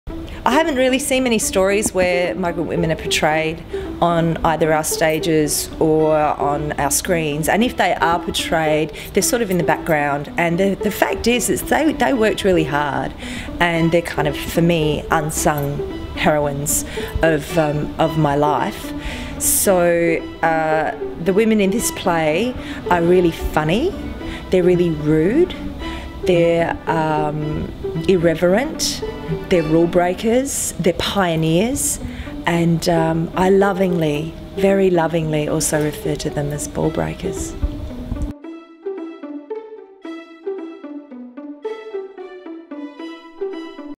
The Gods of Strangers: Big, bold women take the stage
Writer Elena Carapetis tells us about the incredible women from her life that have inspired the characters in The Gods of Strangers.